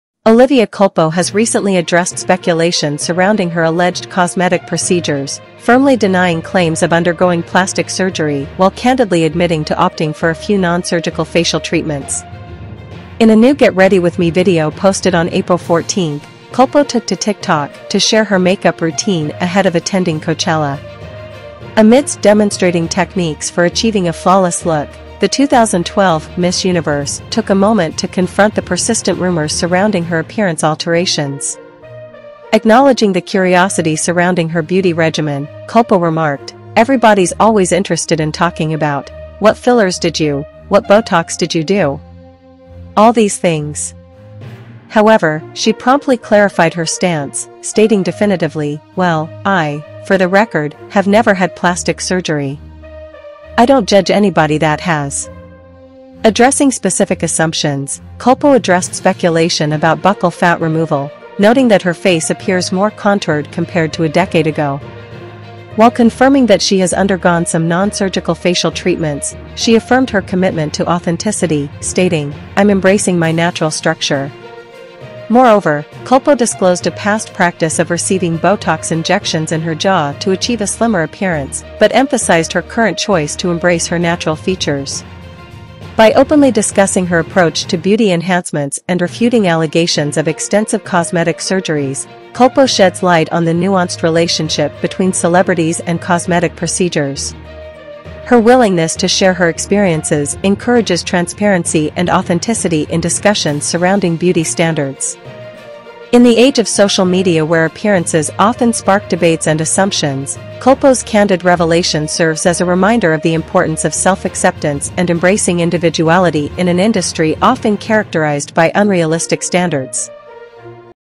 Olivia Culpo Clears the Air: No Plastic Surgery, Just a Few Non-Surgical Treatments!
In her latest "Get Ready With Me" video, Olivia Culpo addresses rumors swirling about her undergoing plastic surgery. Despite speculation, the model confesses she's never had any plastic surgery, but admits to a few non-surgical facial treatments. Catch Olivia sharing makeup tips for a flawless Coachella look while setting the record straight on cosmetic procedures. Don't miss out on her candid revelations and journey to embracing her natural beauty! Watch now!

Timestamps:
0:00 Introduction
0:30 Makeup Tips for Coachella
1:15 Addressing Plastic Surgery Rumors
2:00 Embracing Natural Beauty
3:00 Conclusion & Subscribe!